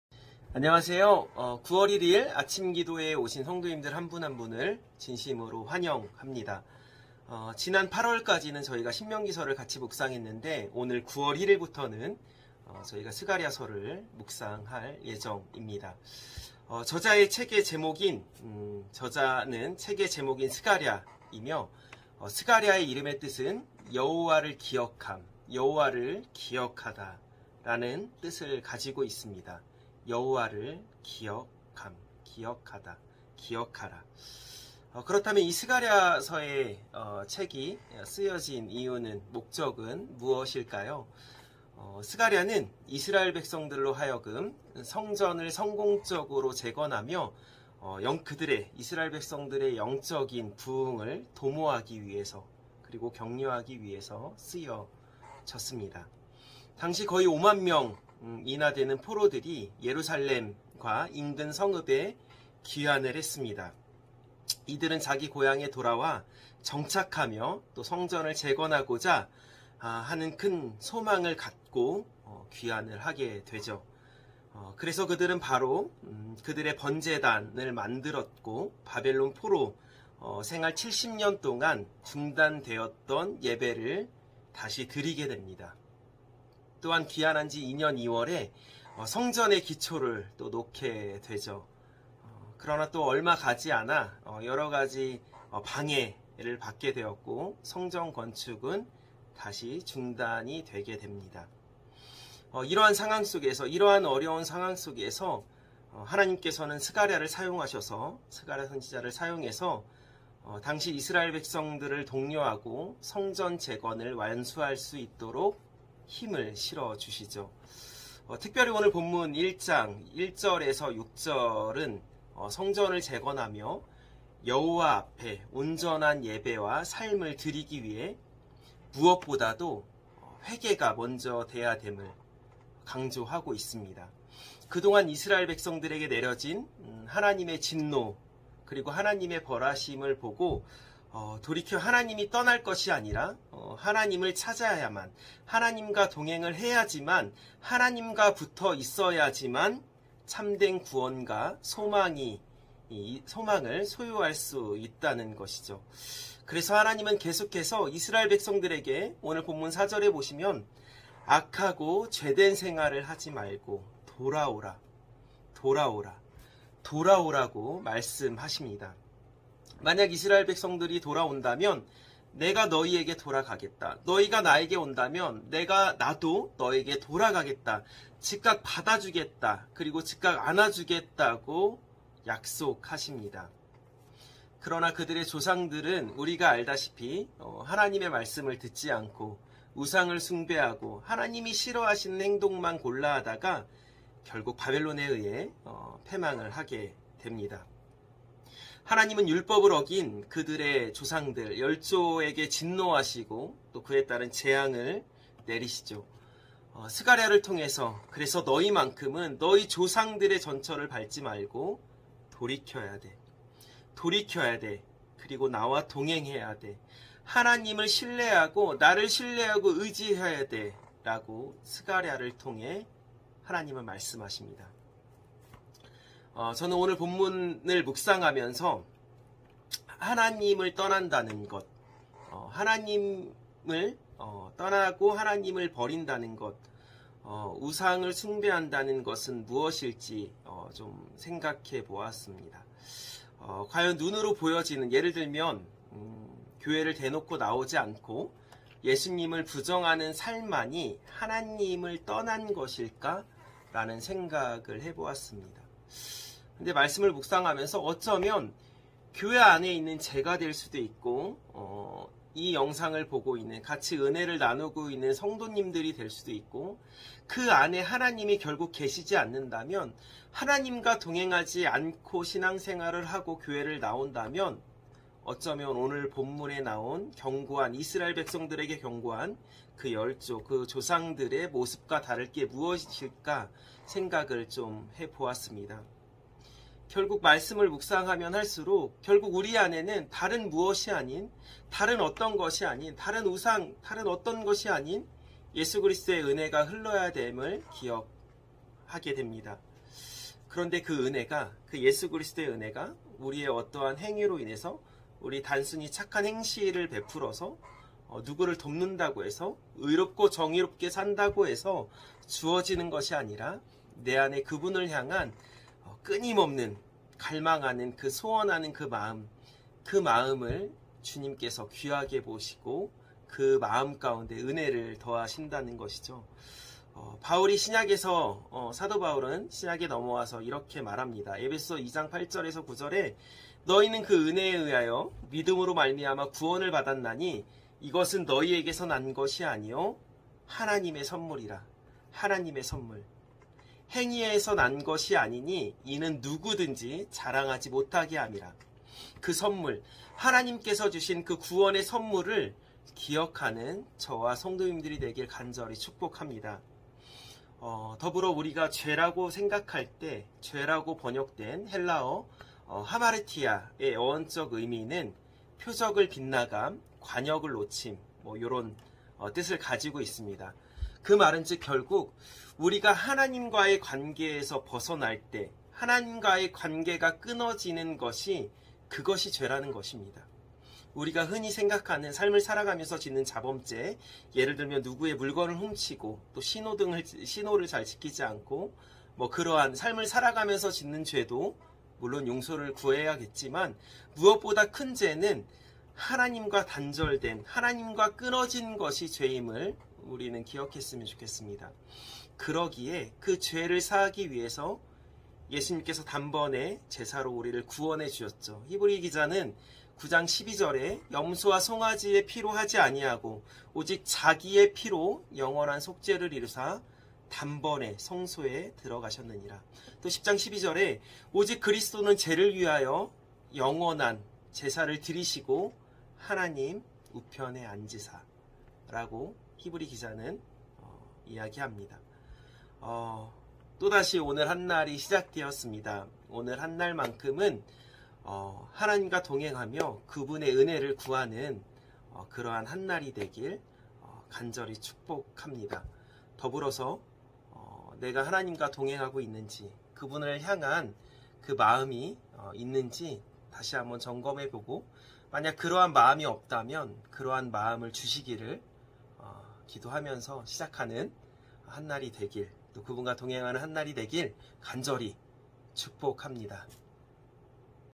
[아침N묵상] 9월 1일(금) | 와플터치QT | 스가랴 1:1-6 (이선학)
1:1 페르시아의 다리우스황제 2년 8월에 여호와께서 잇도의 손자이며 베레갸의 아들인 예언자 스가랴에게 말씀하셨다.

1:2 '나 여호와는 너희 조상들에게 대단히 분노하였다.

1:3 그러므로 너는 백성들에게 전능한 나 여호와가 이렇게 말한다고 일러 주어라. '너희는 나에게 돌아오라. 그러면 내가 너희에게 돌아가겠다. 이것은 전능한 나 여호와의 말이다.

1:4 너희는 너희 조상들과 같이 되지 말아라. 오래 전에 예언자들이 그들에게 악하고 죄된 생활을 하지 말라는 나 여호와의 말을 전하였으나 그들은 듣지 않고 내 말에 순종하지 않았다.

1:5 너희 조상들과 예언자들은 지금 살아 있지 않지만

1:6 나는 그들에게 내 종 예언자들을 통하여 명령하고 경고하였다. 그러나 그들이 그것을 무시하다가 형벌을 받은 다음에야 뉘우치고, 전능한 나 여호와가 작정한 대로 그들이 당연히 받아야 할 형벌을 내린 것을 인정하였다.'